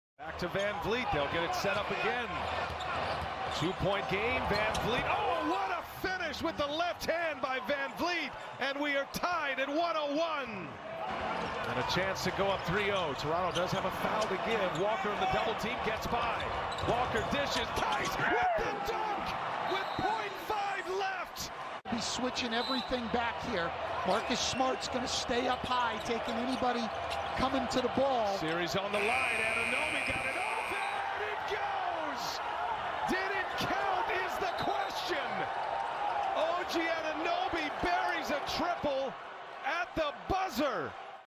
The Last 30 Seconds Of RAPTORS-CELTICS | NBA Playoffs 2020
THE LAST 30 SECONDS OF RAPTORS-CELTICS.
With the Toronto Raptors' season on the line, OG Anunoby rose to the occasion, delivering a last-second shot to steal Game 3.